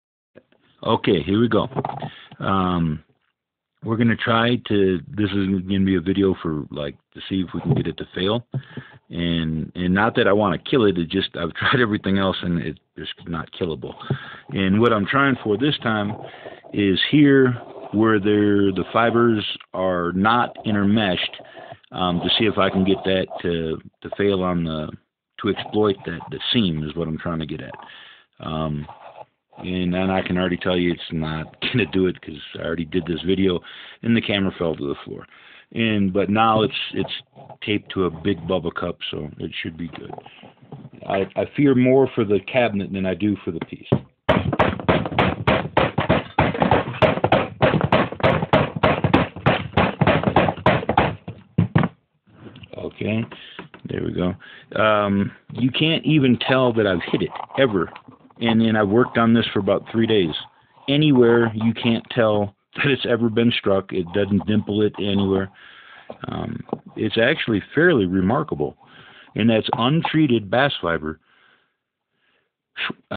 Hemp Bast fiber composite tube, body panel
Trying to wear a hemp bast fiber tube I made thin, to exploit any weaknesses.. not working as planned..  = )